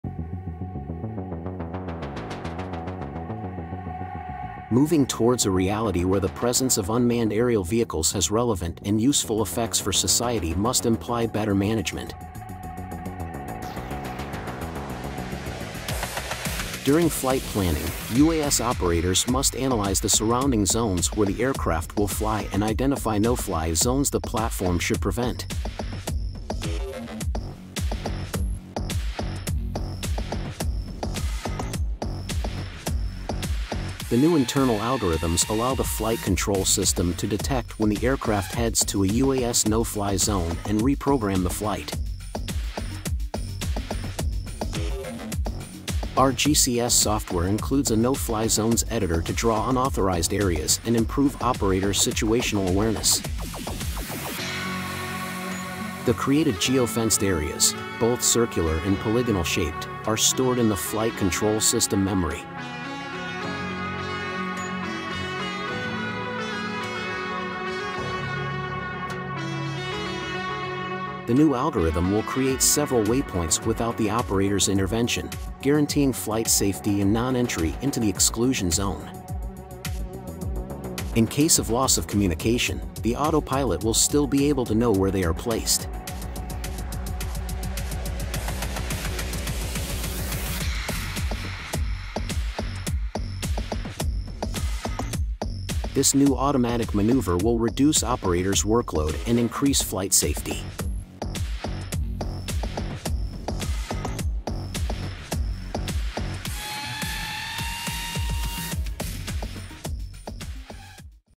Automatic Geofence Avoidance for Unmanned Aerial Vehicles
Geofence capabilities become a key feature inside a reality where the presence of unmanned aerial vehicles has relevant and useful effects for society must imply better management of the airspace.
Flying a UAS requires big responsibilities related to guidance, navigation and control tasks and ensuring their correct use, preventing crossing into military air space or protecting and defending personnel, facilities, and assets. Protecting everyone in the air and on the ground is very important.
During flight planning, before each flight, UAS operators must analyze the surrounding zones where the aircraft will fly and identify hot spots the platform should avoid. These are what in aviation are called unauthorized fly zones or no-fly zones.
No-fly zones have traditionally been categorized into two levels: restricted operations and excluded areas. Restricted areas are limited to authorization by the competent agency where the operation is performed and is subordinate to fulfilling the conditions imposed in these zones (airports, heliports, national parks, military installations, hospitals, and nuclear power plants). On its part, excluded areas are zones where the flight is prohibited. These areas are normally associated with military installations, war zones, etc.

- UAV Navigation-Grupo Oesía GCS Software Geofence Editor
Visionair, UAV Navigation-Grupo Oesía’s GCS software, includes a No-Fly Zones Editor to draw the No-Fly Zones on the mission planning map to improve operator situational awareness and prevent the aircraft from entering unauthorized areas.
The created geofenced areas, both circular and polygonal-shaped, are stored in the autopilot; hence, in case of loss of communication, the Flight Control System will still be able to know where the not-permitted zone is.
Once the no-fly zone is defined, Visionair allows the operator to configure the execution of different actions regarding those areas: an automatic parachute release, the self-activation of hover mode in the case of rotary wing platforms, and the new automatic mission replanning.

- Advanced Algorithms for Intelligent and Automatic Mission Replanning
The new internal algorithms allow the flight control system to detect when the aircraft heads to a UAS No Flight Zone and reprogram the flight to keep the unmanned aircraft away from that area. This algorithm will create a new flight path without the operator’s intervention, guaranteeing flight safety and non-entry into the zone of exclusion. This automatic maneuver will reduce the workload of operators and increase flight safety.
With this new functionality, the UAS will be safe in the event of a wrong flight plan where one of the legs of the flight plan intercepts a no-fly zone. The autopilot will autonomously execute the required maneuvers to reach the following waypoint without entering the area, allowing it to continue the mission. In the same way, if the designated UAS operator commands an erroneous heading, the system will recalculate the route, avoiding the geofenced area.
UAV Navigation-Grupo Oesía continues to work with a focus on flight safety. Entry into a restricted or excluded airspace may endanger the operation or, on top of this, compromise manned aviation flight safety, populations, or infrastructures. Therefore, relying on an advanced and intelligent flight control system capable of preventing risks becomes essential.